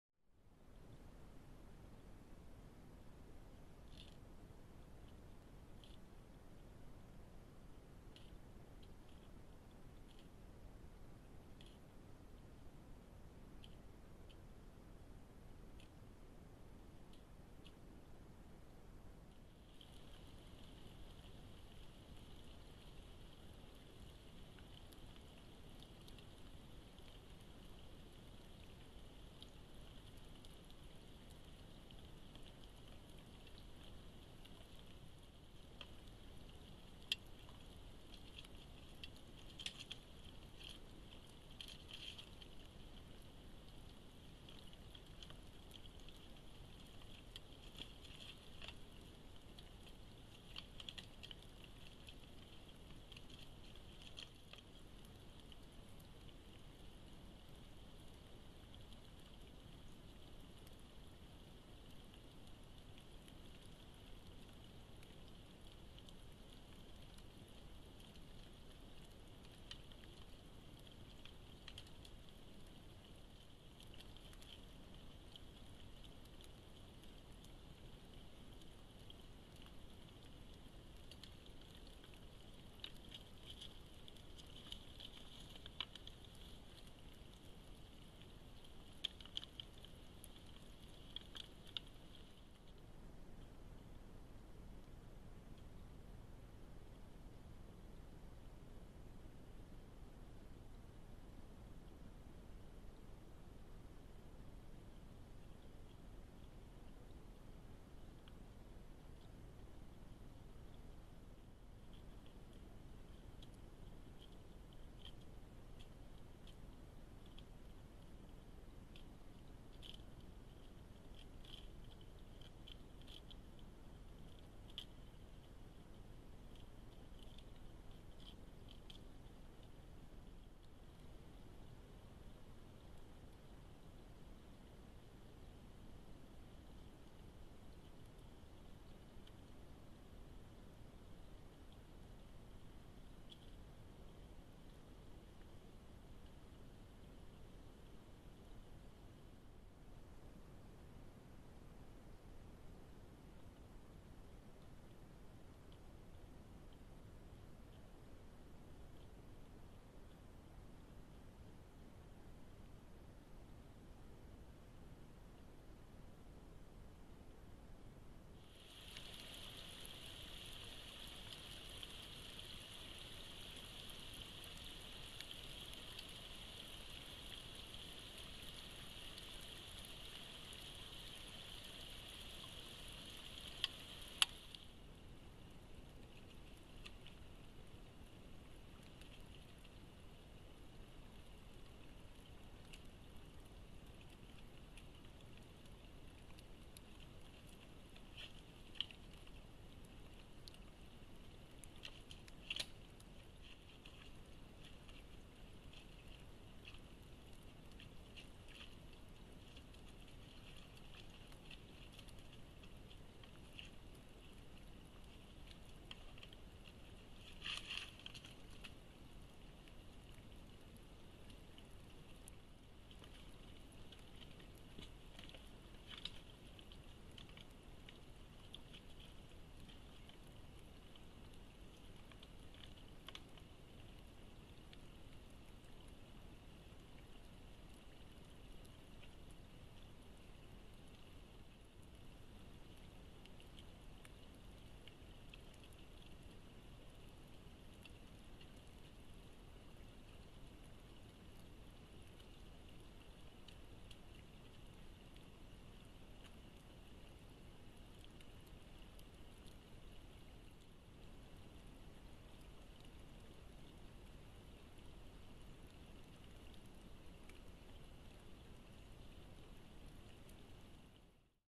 Licking Branch Sept 2021
Sitka blacktail deer at the end of summer 2021